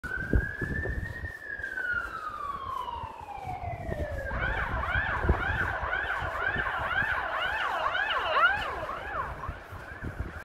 Finnish fire truck responding with very visible blue lights & loud siren | Paloauto hälytysajossa
Blue lights are very visible during the day and night.

Lapin pelastuslaitoksen paloauto kiitää tehtävälle. Siniset valot erottuvat erittäin hyvin niin pimeällä kuin päivänvalossakin.